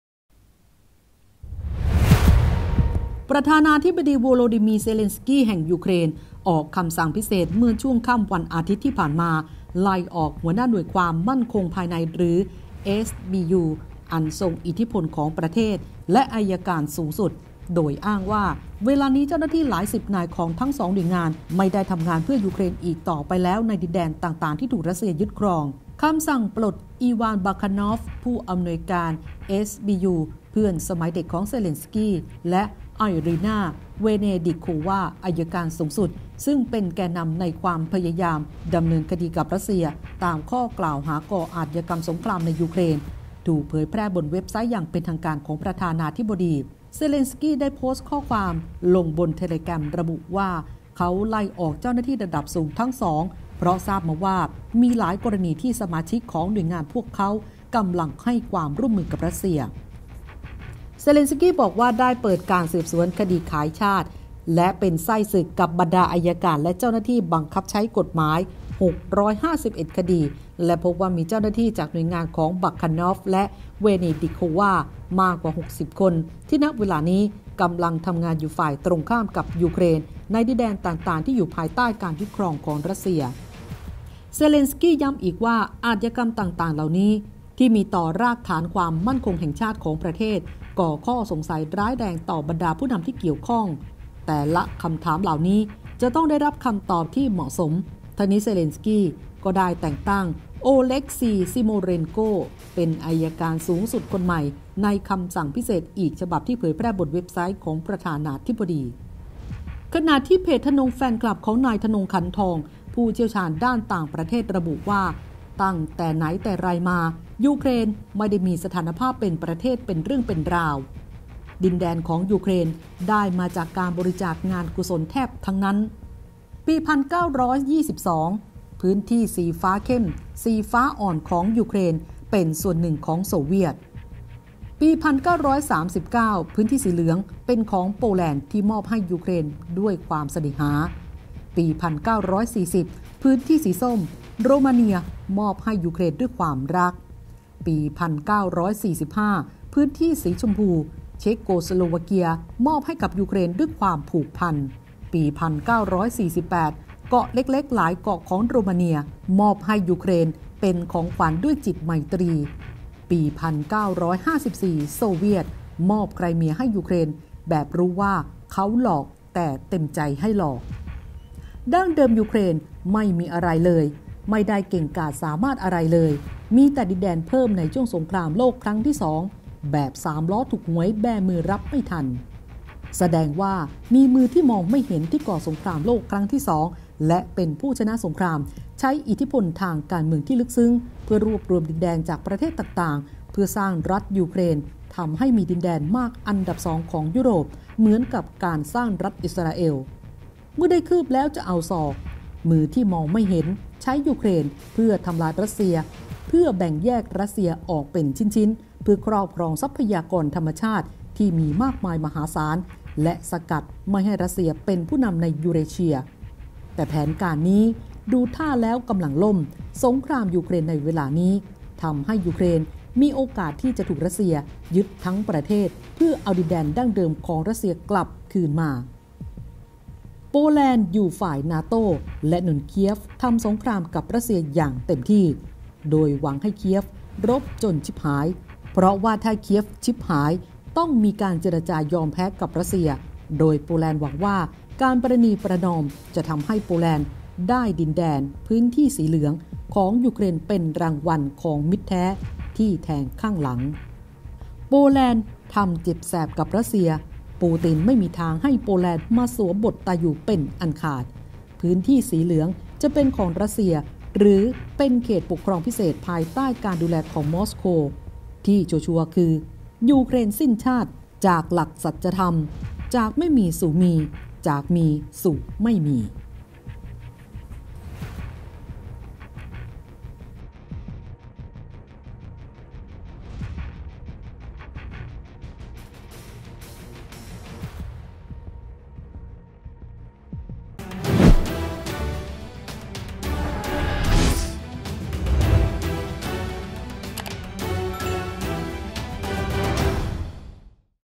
"ยูเครน"ระอุ "เซเลนสกี" ไล่ออก เพื่อนสนิท"ผอ.ข่าวกรอง" พร้อม"อัยการสูงสุด" | ข่าวเด่น | TOP NEWS
การเมือง"ยูเครน"ระส่ำ "เซเลนสกี" เซ็นคำสั่ง"ไล่ออก"เพื่อนสนิท พ้นตำแหน่ง"ผู้บัญชาการสำนักงานรักษาความปลอดภัยและข่าวกรองแห่งชาติ" พร้อม"อัยการสูงสุด" อ้าง ตรวจพบ สมาชิกหลายคน กำลังให้ความร่วมมือกับ"รัสเซีย" ด้าน "นักวิชาการ" วิเคราะห์ ถึงคราว "ยูเครน" สิ้นชาติ จากหลักสัจจธรรม"จากไม่มีสู่มี จากมีสู่ไม่มี" เชื่อ "ปูติน" ไม่ยอม"โปแลนด์"สวมบทตาอยู่ ชิงดินแดนภายใต้การดูแลของ"มอสโก"